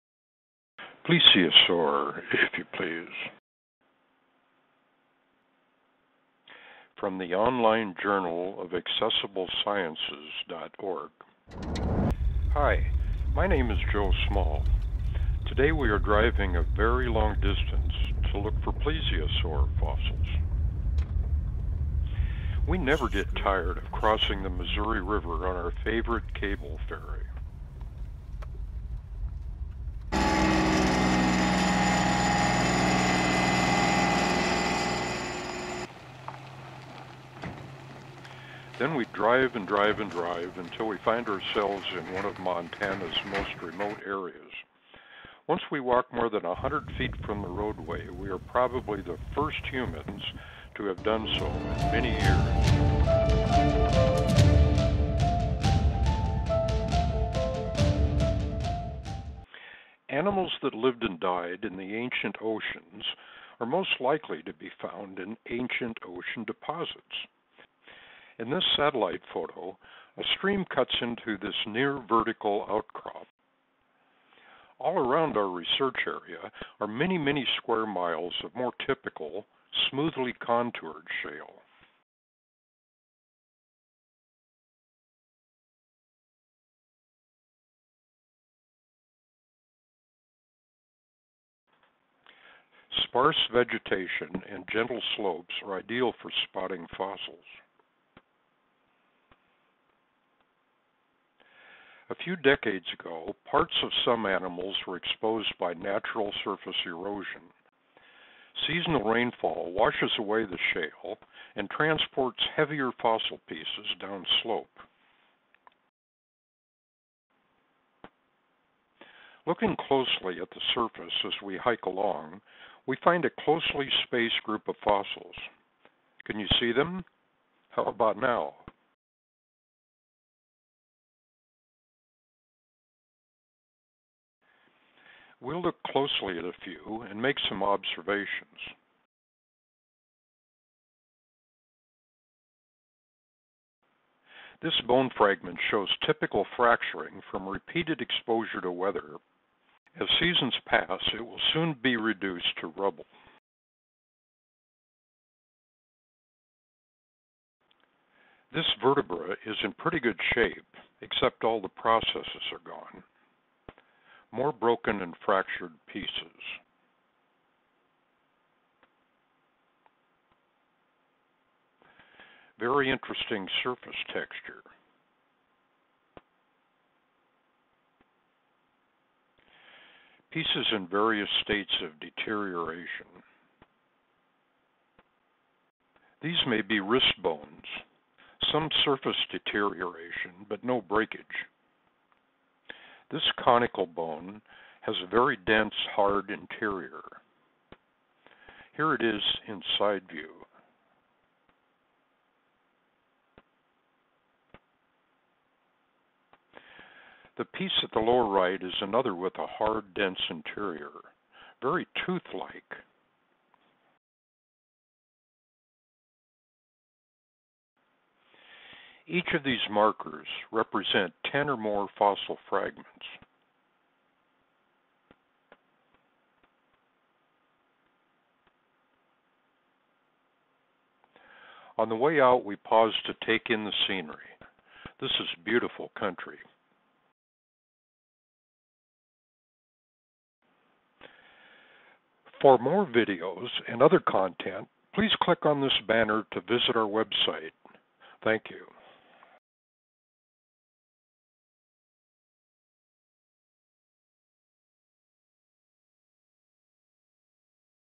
Plesiosaur, If You Please
Finding Plesiosaur fossils in Central Montana.  A production of JournalOfAccessibleSciences.org